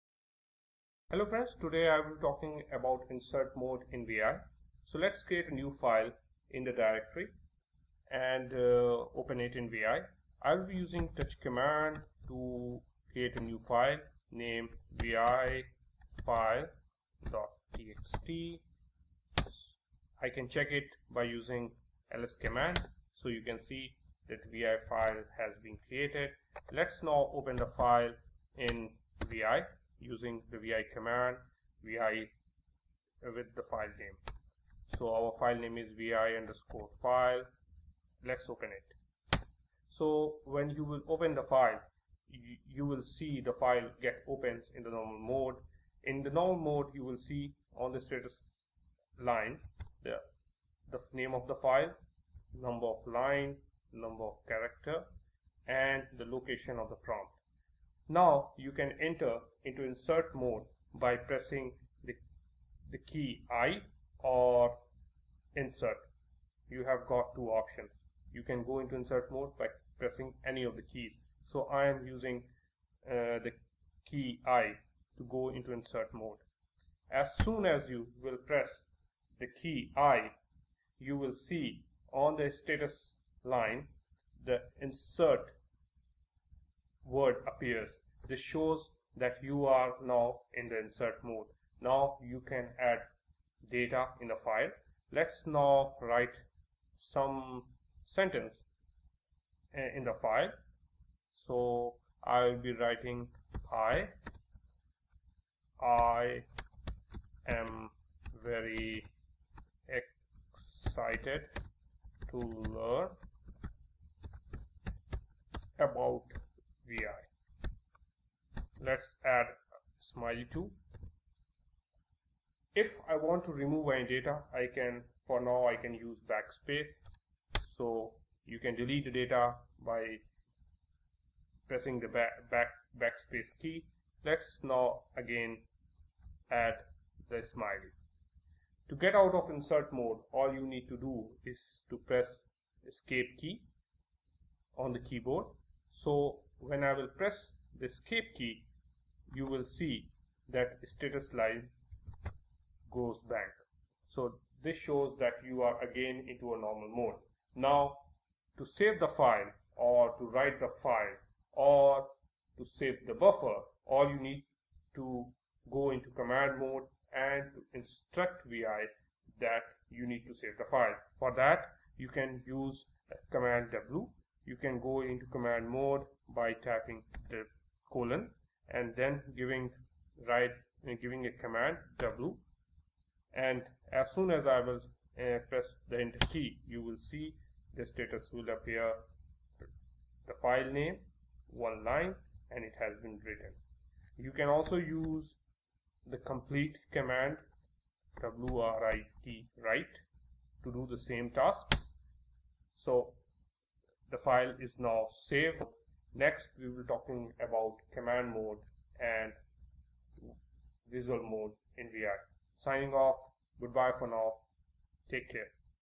Insert mode in vi / vim (vi for beginners Part 5)
Small lecture about the insert mode in vi or vim editor.